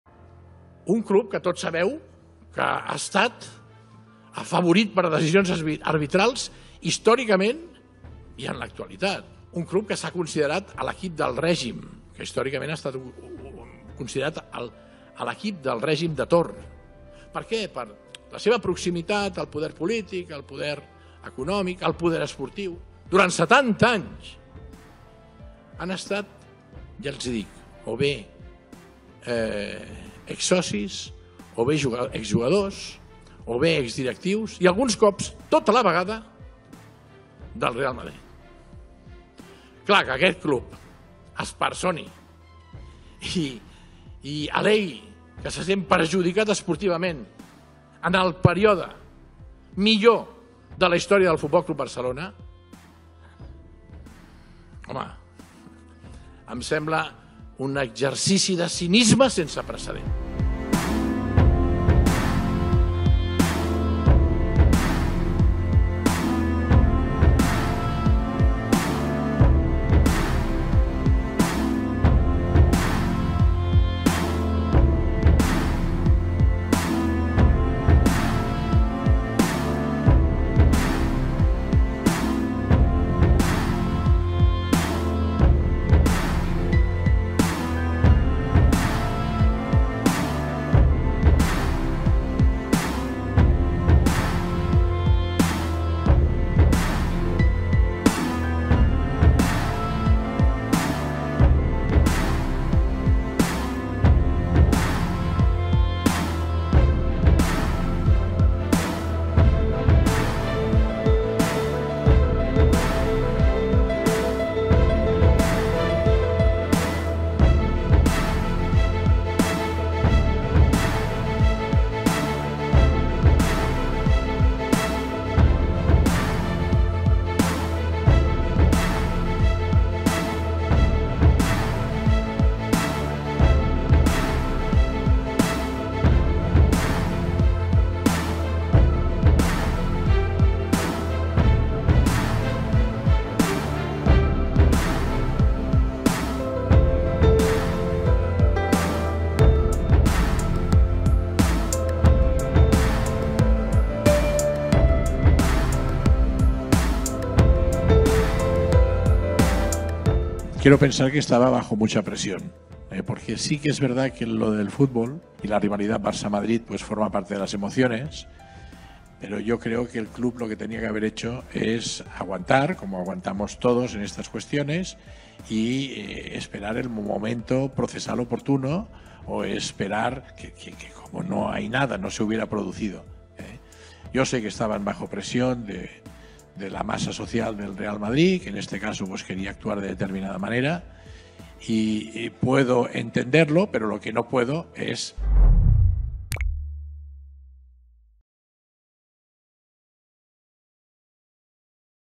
ناد نعرفه جميعا كان مفضلا لدى الحكام.. شاهد قذائف لابورتا النارية تجاه ريال مدريد وفلورنتينو بيريز
فتح خوان لابورتا رئيس برشلونة النار على عريمه ريال مدريد خلال المؤتمر الصحفي الخاص بالرد على اتهام النادي في قضية خوسيه نيجريرا، النائب السابق لرئيس اللجنة الفنية لحكام الليجا.